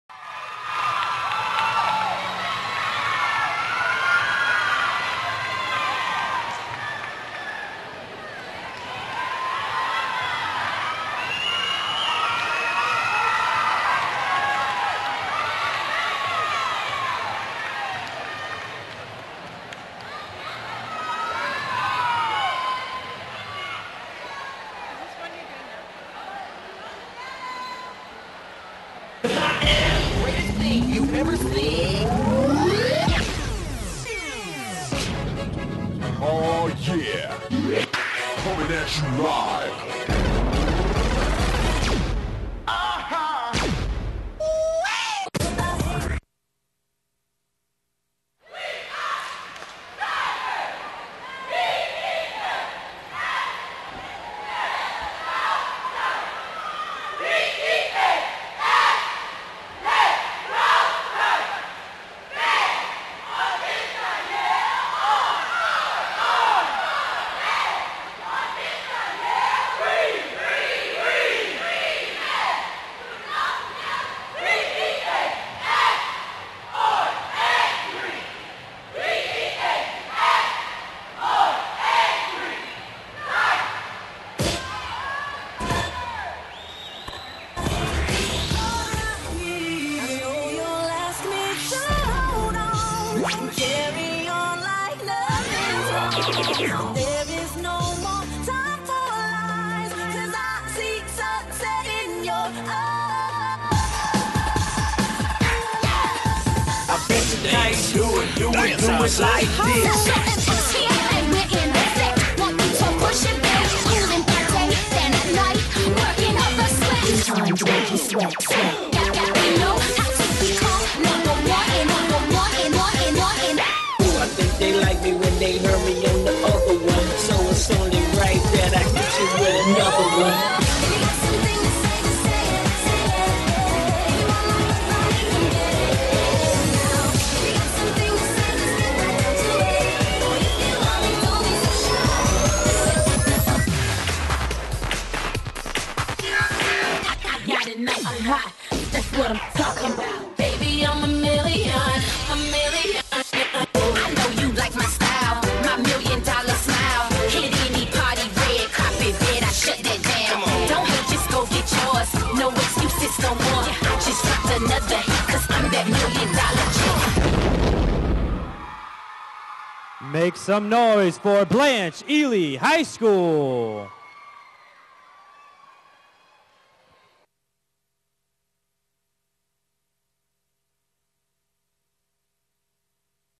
Blanche Ely High's Large Non-Tumbling Team Preliminary Performance
Blanche Ely High School's routine from the FHSAA Division 2A Large Non-Tumbling Competitive Cheer Championship Finals from Silver Spurs Arena in Kissimmee, Florida.